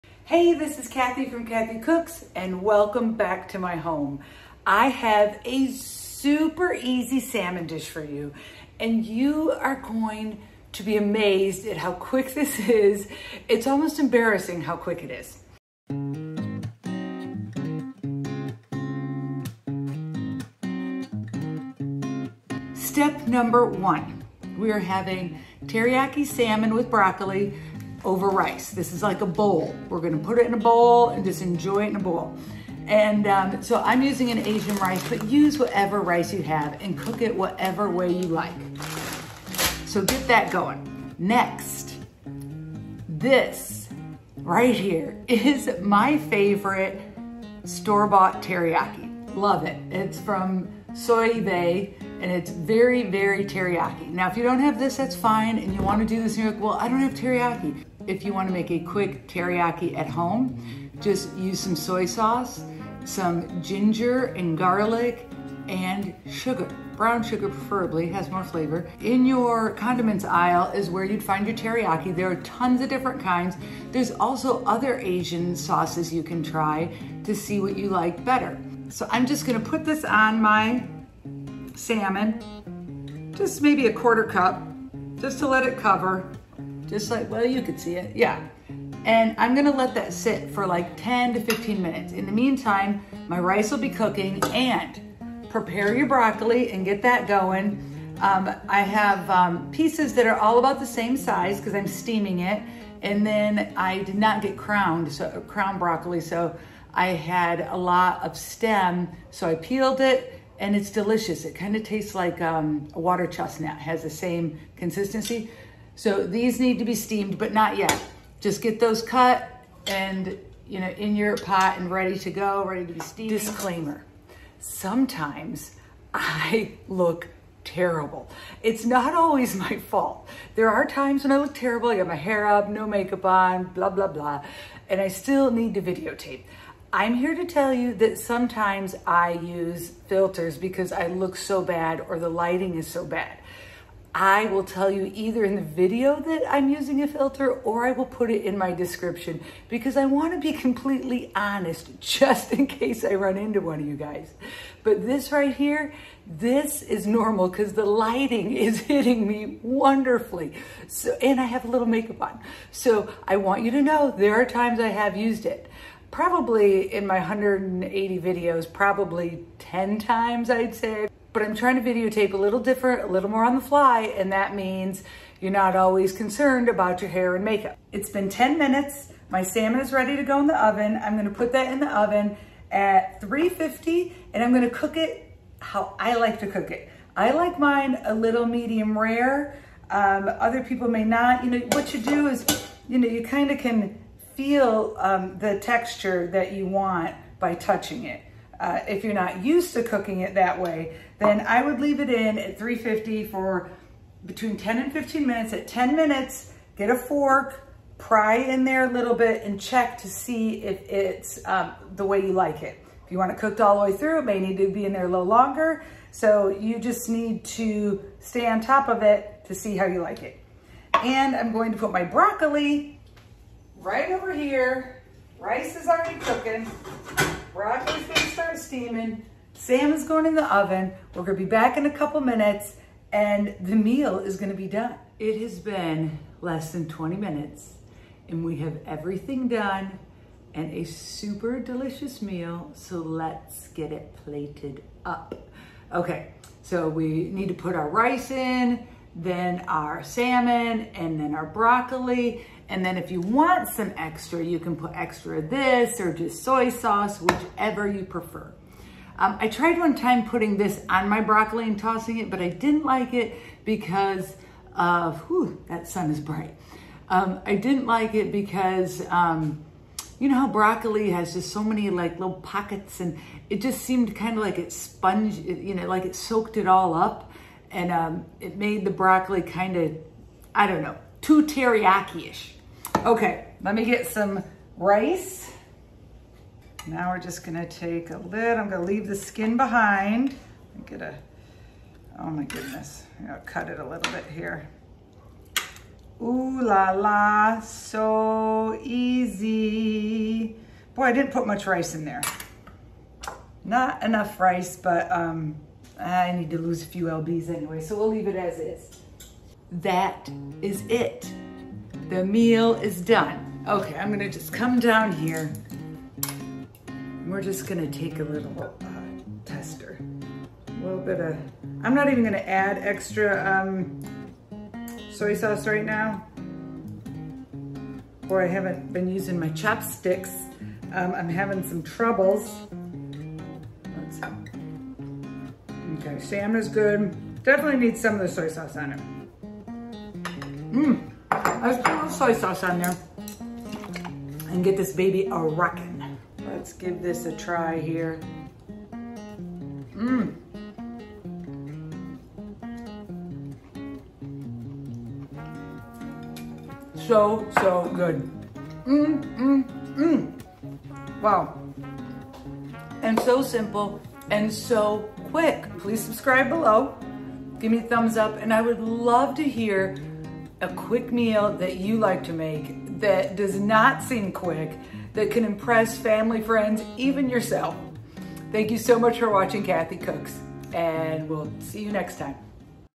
Stop wasting time in the Kitchen! Quick and Easy Teriyaki!
Spending too much time in the kitchen?  Watch this video and learn this easy Salmon and Broccoli Teriyaki.  You can not get a simpler meal than this one!!  It is so simple that I am not leaving a recipe!!!

ALSO the music on this video was composed by my son Max Carlson, check out his album on spotify.

00:00 Introduction
00:23 Marinate and cutting veg
02:03 Filter Disclaimer
02:57 Cooking time
03:55 Taste Test
05:49 outro